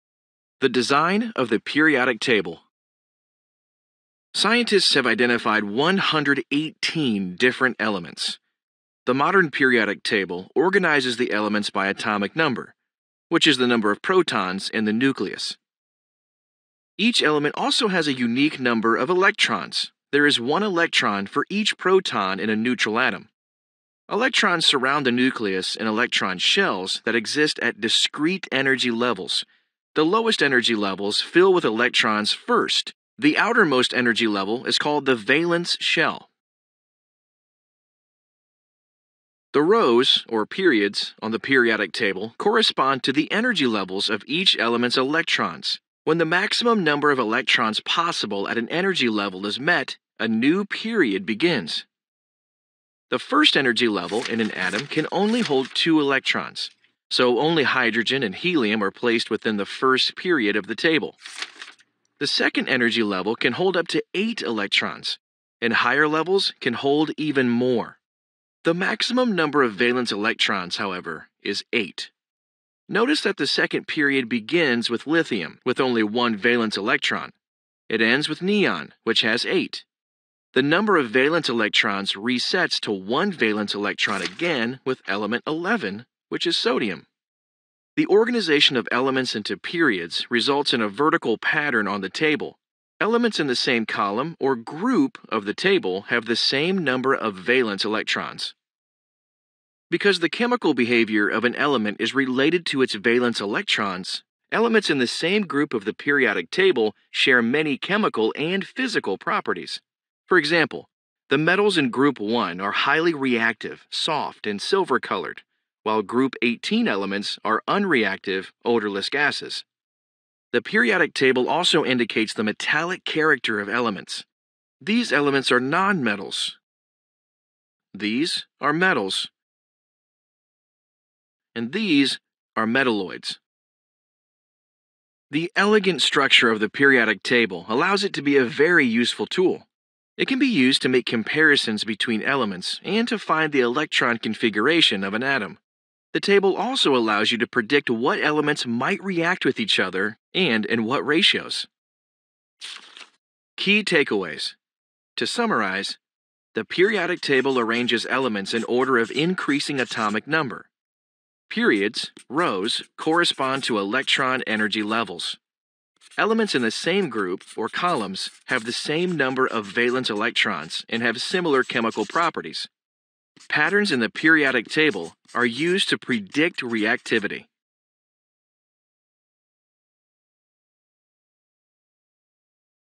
The Design of the Periodic Table
This video relates the structure of the periodic table to the varying structures of the chemical elements, and is related to Sections 5.1 and 5.2 of the course textbook.